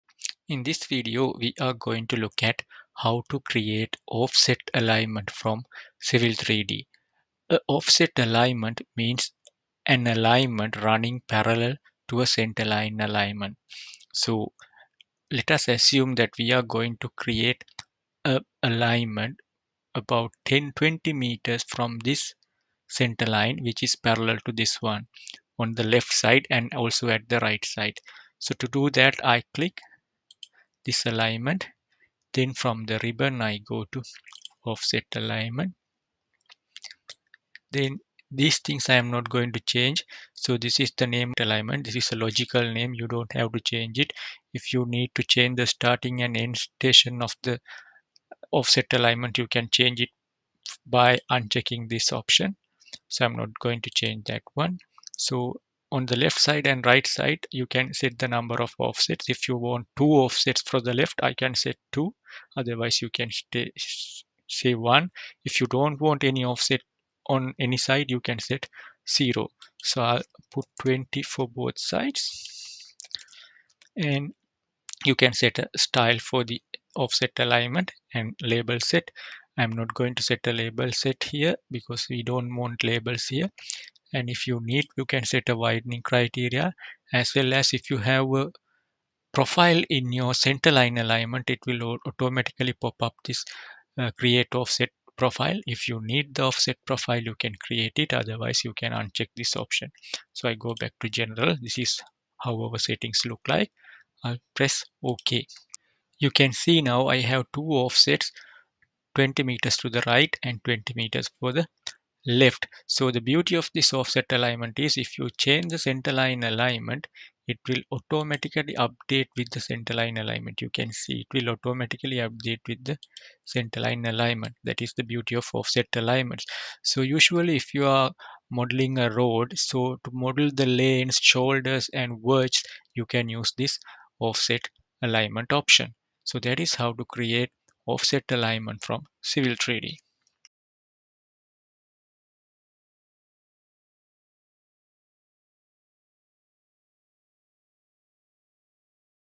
Create Offset Alignment in Civil 3D
In this video we are going to look at how to swap edges in Civil 3D surfaces to get more accurate surface. So I have a surface on my screen so I click on the surface and go to object viewer and check about my surface. You can see there is some kind of error in this surface. This is supposed to be here like a complete channel or something or curb return. So this corner should be at this level and the top should be flat. So there is some error in the surface. How do we need to correct that one there? This is where we need swap edges in slope surfaces. So I close this one South. Sometimes to do this you need to turn on a style that shows triangles in your surface. So to do that you select the surface, right click surface properties. So make sure you have a style surface style which represent which shows contours and triangles. In this case contours and triangles. Whatever the case it should show the triangles. So I close this one. So what I do is I'll go to here top and I make it SW isometric. And from the 2D wireframe I'll make this conceptual. So now we can see this error in a 3D mode. So if you have a low end computer and if you have a larger surface this might be a very hard task. But if you have a good high end computers you this can can be done easily without
 lagging in your computers. So select your surface, go to edit surface, go to swap edge. And zoom into this erroneous area. This is the triangle where we need to correct to change the shape of the surface or the swapping edges. So we'll click this one and click here change surface so it will automatically swap the edge. Now you can see I'll click escape now. Now you can see our surface is correct that you can see it. Properly. So after doing the corrections you can make this 2D wireframe and this one top and if you need you can go back to the object viewer and verify now your surface is correct. So if you have more errors in the surface you can correct this correct in the same manner.